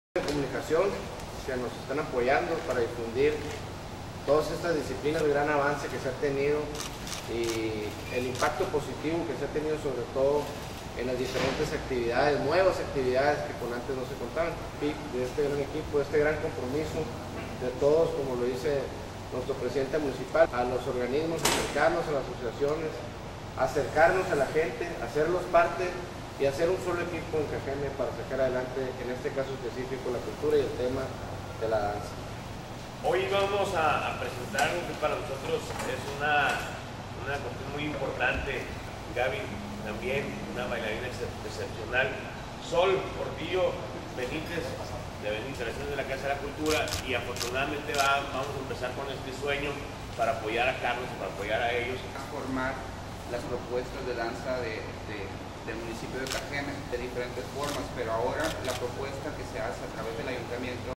Escuela Municipal de Danza de Cajeme EMUDEC
Con el fin de potenciar la actividad dancística en jóvenes interesados por desarrollar habilidades y aptitudes artísticas, el Gobierno municipal de Cajeme impulsa la Escuela Municipal de Danza de Cajeme (EMUDEC).
En el vídeo, Emeterio Ochoa, Secretario de Desarrollo Social de Cajeme.